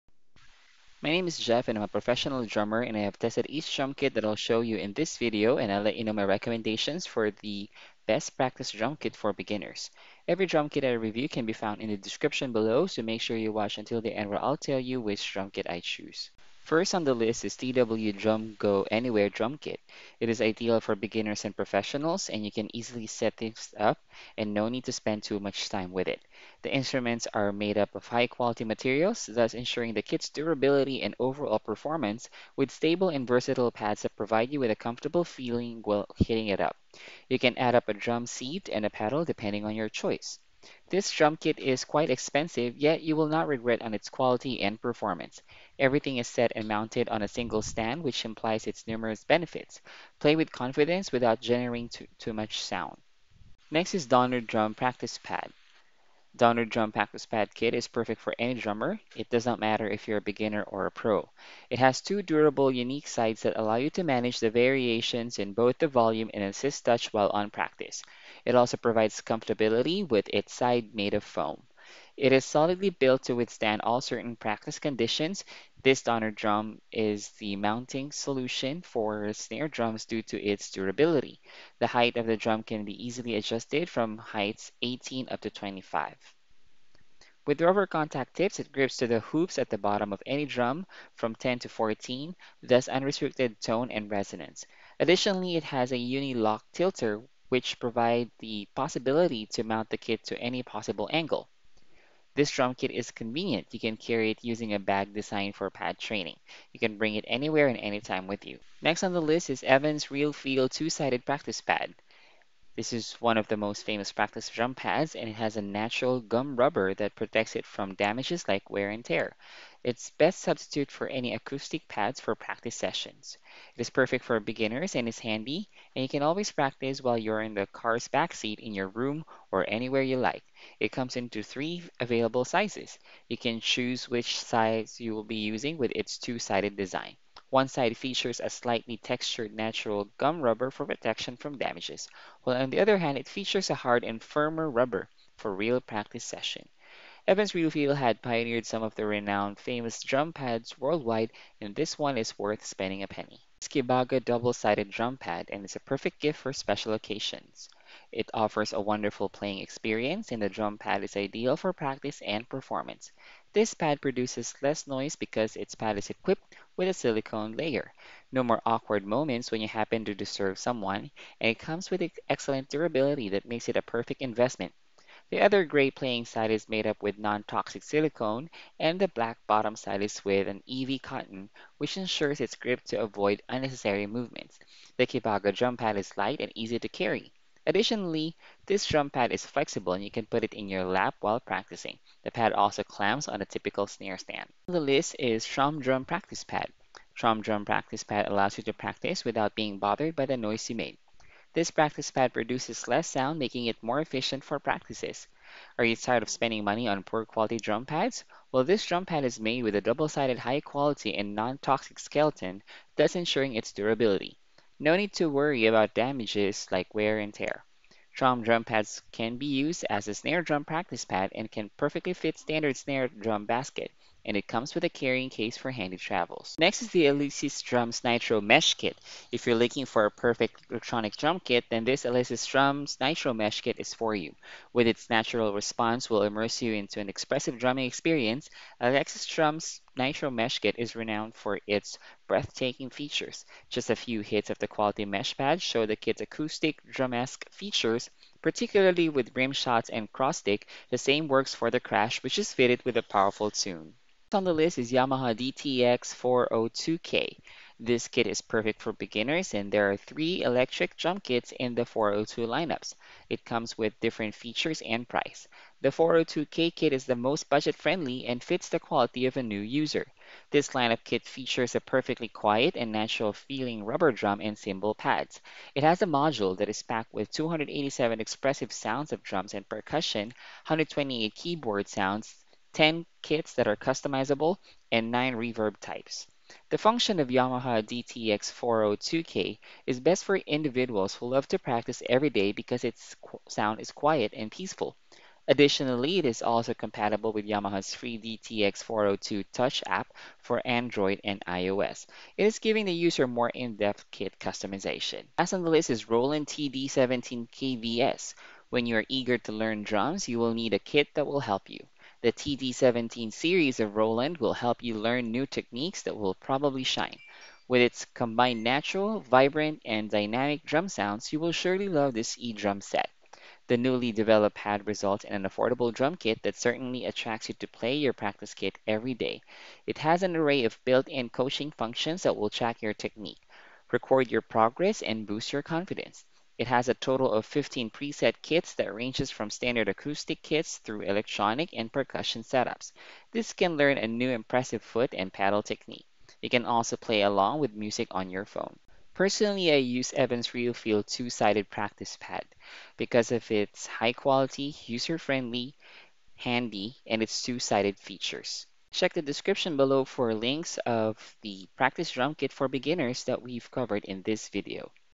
Best Practice Drum Kit for Beginners
Here are the top 8:
1. DW Drum Workshop CPPADTS5 Go Anywhere Pad Set with Stand
2. Donner Drum Practice Pad With Snare Drum Stand Adjustable Kit
3. Evans Realfeel 2-Sided Practice Pad
4. KIBAGA Double-Sided Drum Pad
5. Tromme Drum Practice pad
6. Alesis Drums Nitro Mesh Kit
7. Yamaha DTX402K
8. Roland TD-17KV-S